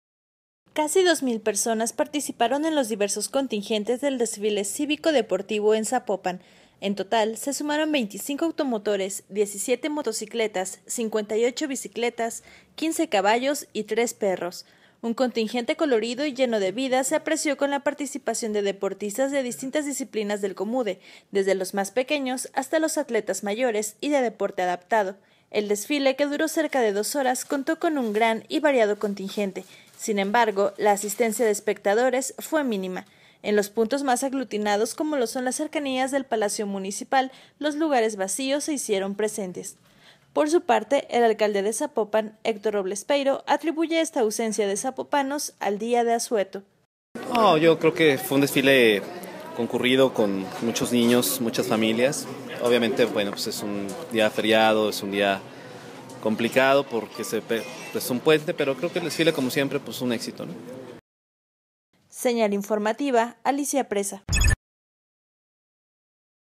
Señal Informativa: Poca asistencia al desfile de aniversario de la Revolución Mexicana en Zapopan
Poca asistencia al desfile de aniversario de la Revolución Mexicana en Zapopan.